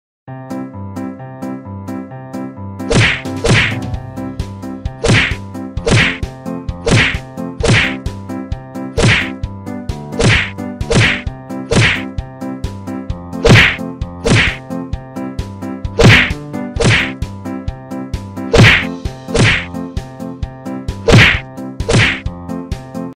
Hercules Beetle🐞- 1 VS 5 -कौन जीतेगा who is gonna win?😱😎
Hercules Beetle - 1 VS 5 who is gonna win? The Hercules beetle (Dynastes hercules) is a species of rhinoceros beetle native to the rainforests of Central America, South America, and the Lesser Antilles. It is the longest extant species of beetle in the world, and is also one of the largest flying insects in the world.